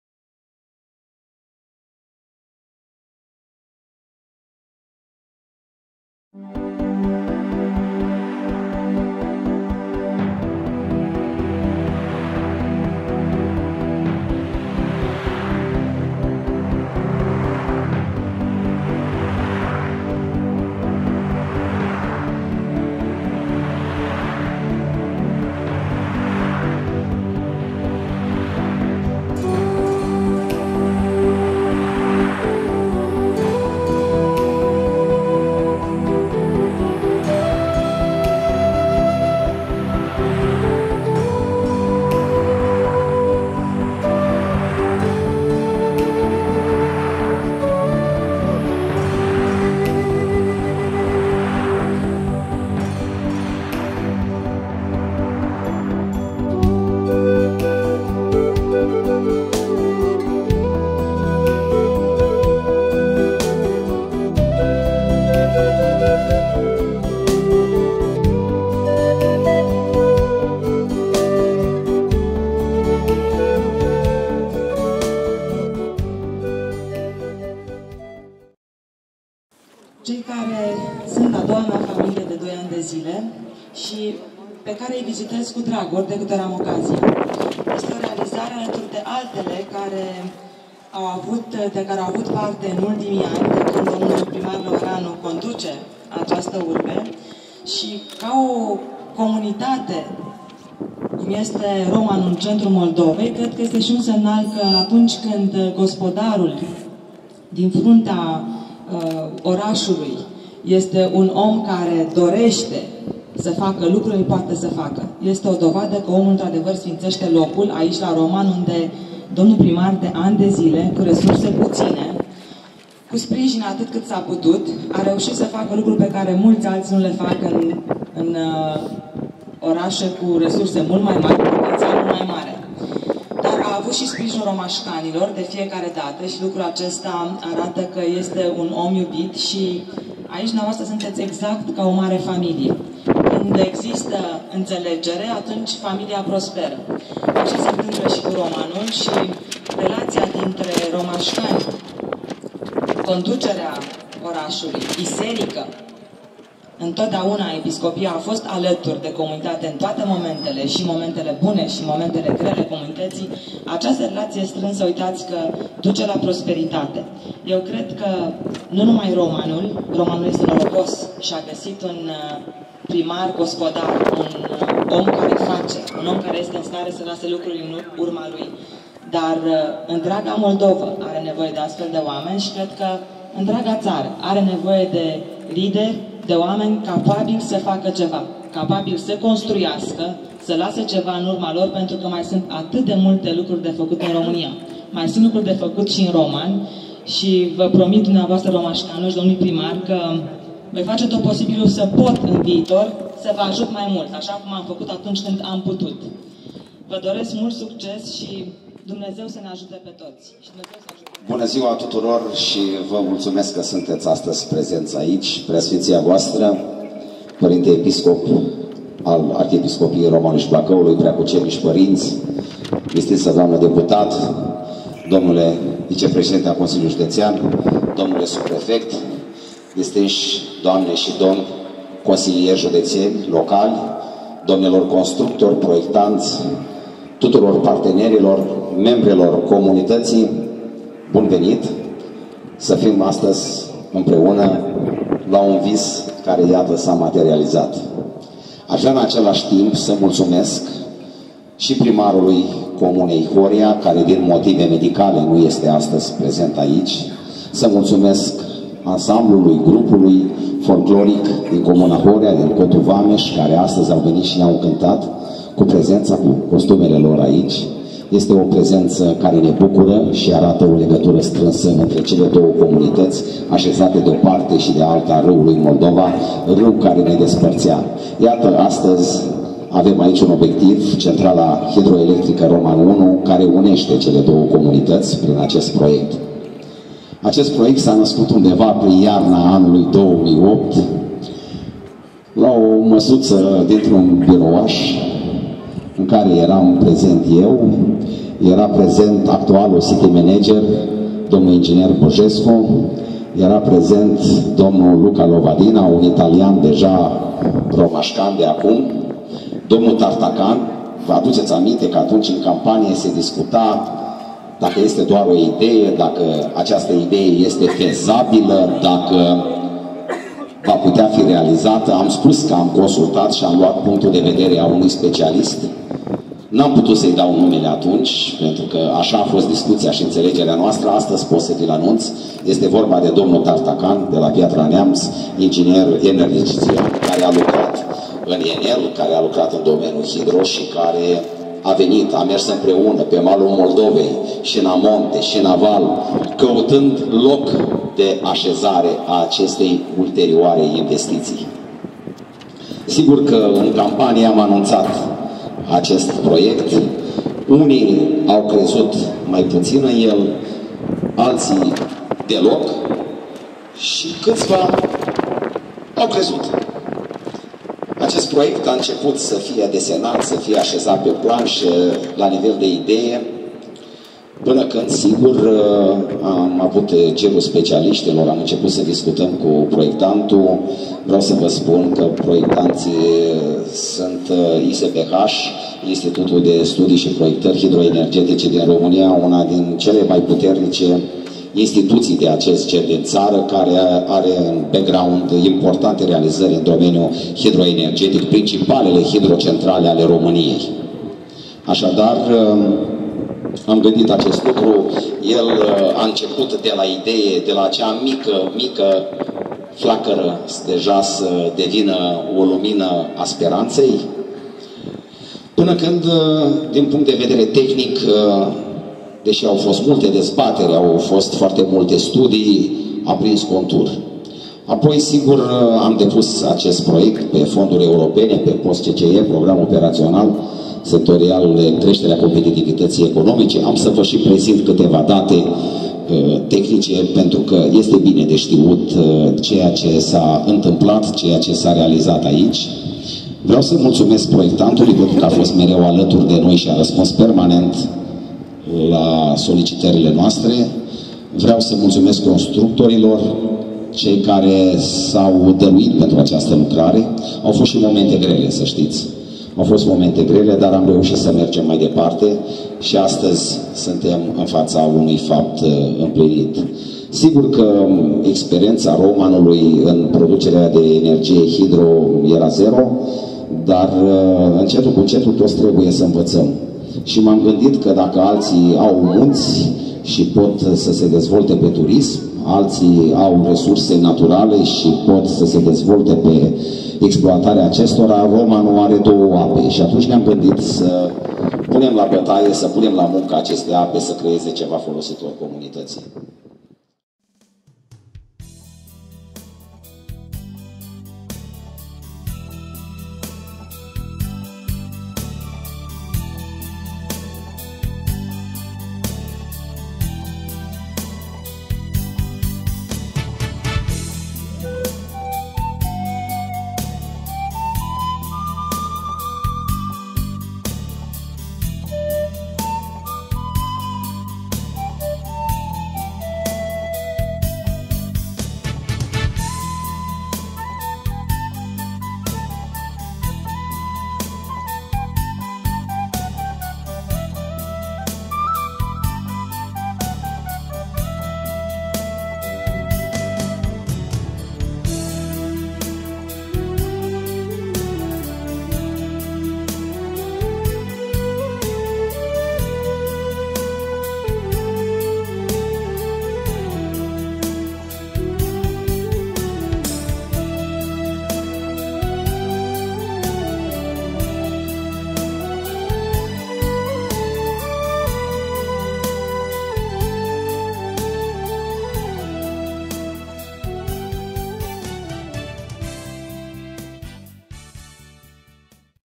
24 09 2014 INAUGURARE CENTRALA HIDROELECTRICA MUNICIPIUL ROMAN ELENA UDREA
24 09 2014 INAUGURARE CENTRALA HIDROELECTRICA MUNICIPIUL ROMAN -  ELENA UDREA - LAURENȚIU LEOREANU